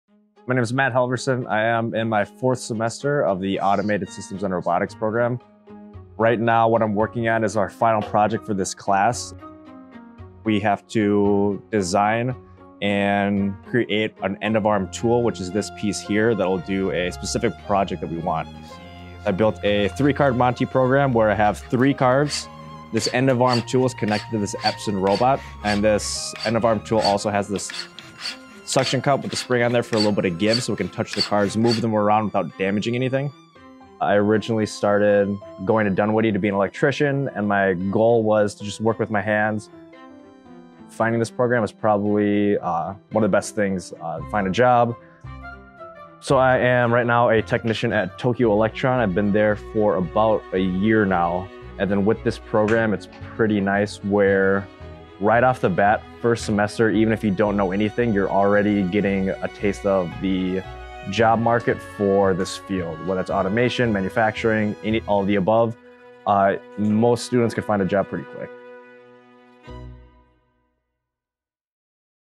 AUTOMATED SYSTEMS & ROBOTICS FINAL PROJECT
We are showing off some of our Automated Systems & Robotics students final projects. Two-years to a degree, and 100 percent job placement.

Music licensed through Soundstripe:
VCJMEBJFVW1PC9TX